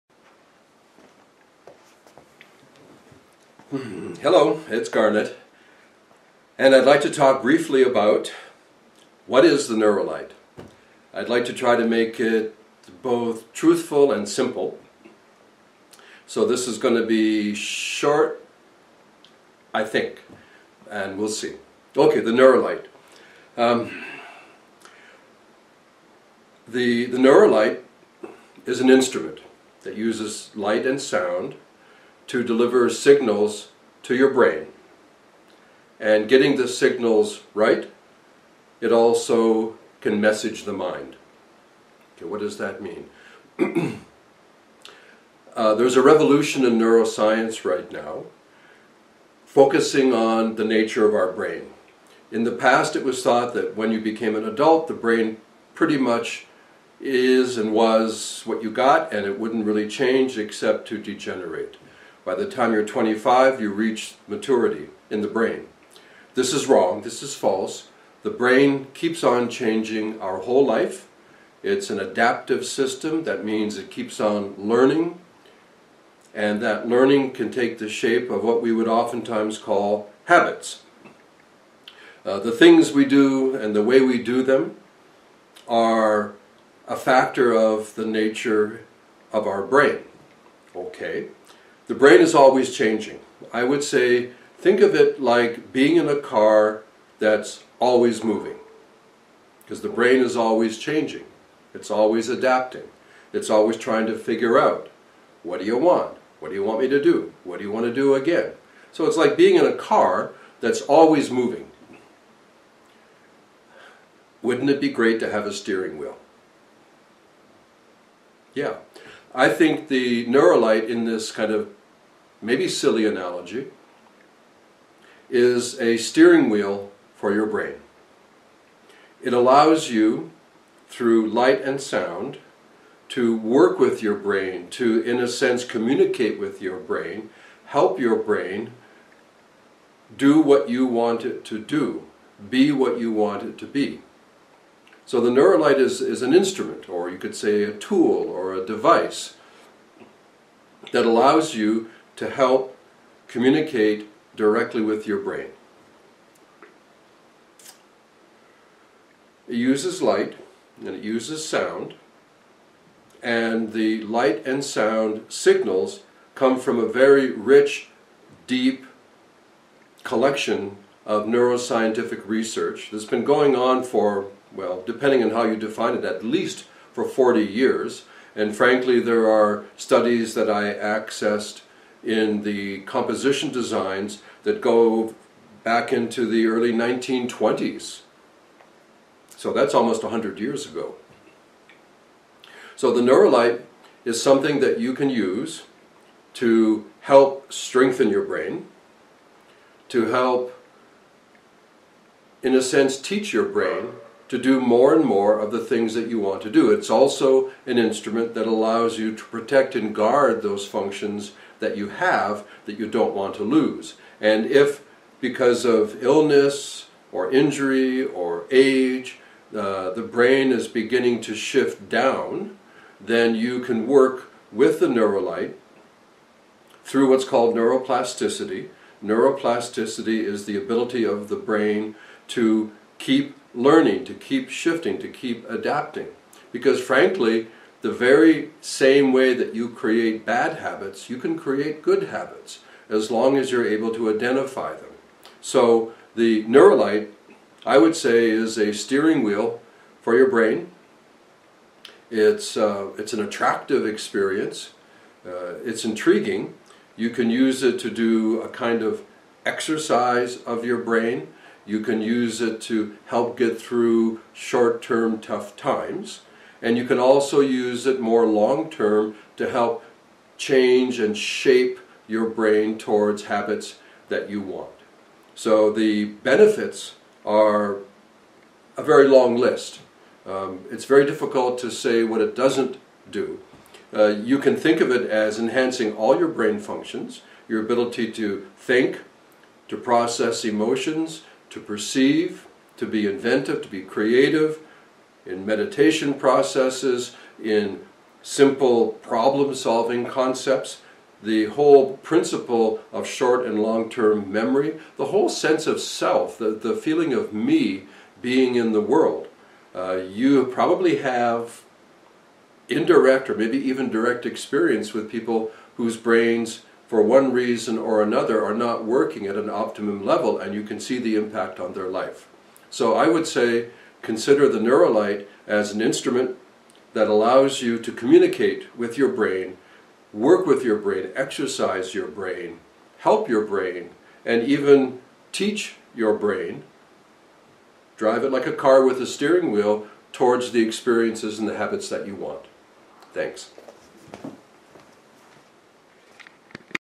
Basic #1 What is the Neuro Light
A short & super simple introduction to the Neuro Light